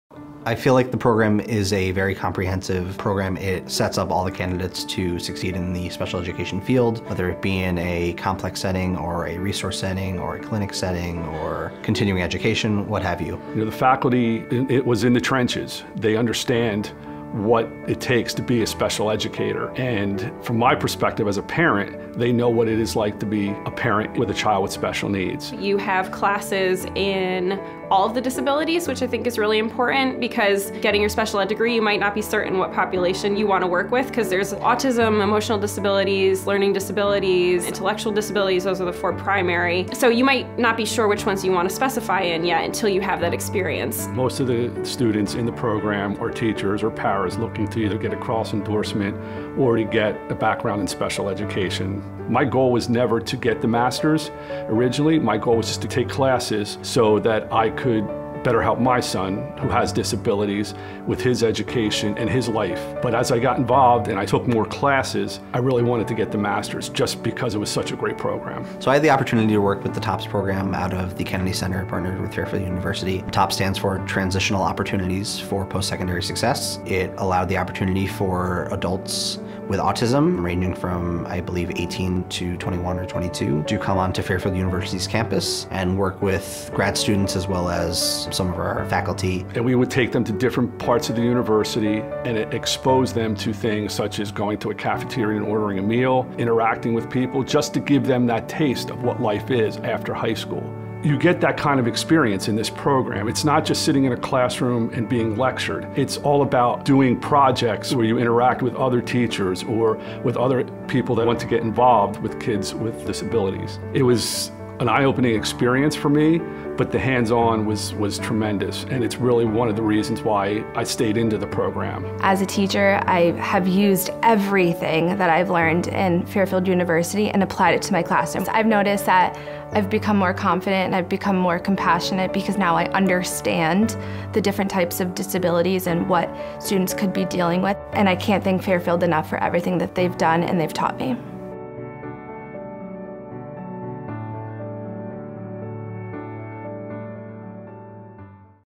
Teachers and Parents Studying Special Education
Fairfield University's Master of Arts in Special Education is dedicated to preparing special educators with the expertise to meet the unique, individualized learning needs of students with a variety of exceptionalities. We are committed to ensuring that our special education teacher candidates are provided with a rigorous program of excellence that is designed to develop your skills and abilities to work with a specialized population of students. The Program accomplishes this by offering a curriculum that provides extensive coverage of a broad range of areas pertaining to disabilities, diagnostic procedures, and related psychoeducational issues.